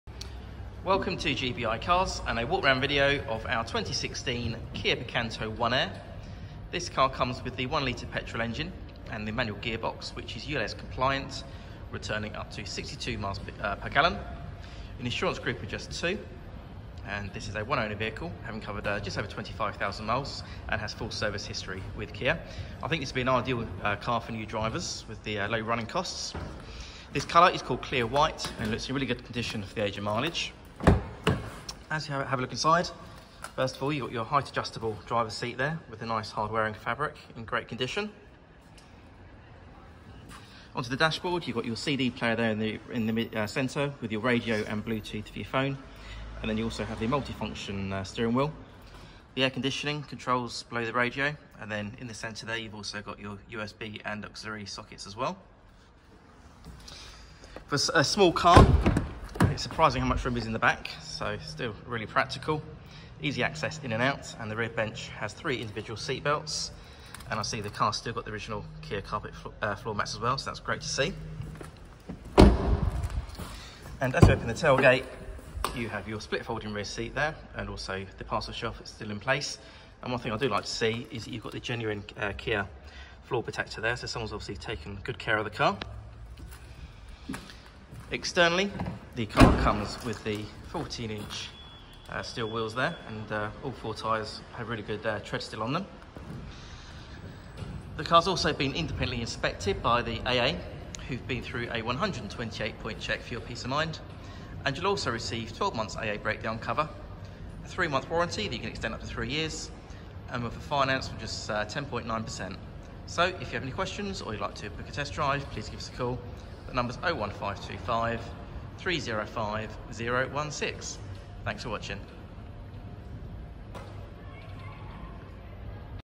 Kia Picanto 1 Air
Our 1 owner Ulez Compliant 2016 Kia Picanto 1 Air has full Kia Service History and a low insurance group of just 2, so an ideal car  for new drivers. Features include Air Conditioning, Bluetooth and a CD player. The car passed its AA Inspection straight away and the MOT is not due until 19/07/2024.The Full Kia Service History is as follows:

28/06/2023 24,221 miles,
11/07/2022 21,347 miles,
19/07/2021 19,300 miles,
06/07/2020 16,934 miles,
15/07/2019 13,389 miles,
16/07/2018 8,539 miles,
17/07/2017 4,308 miles.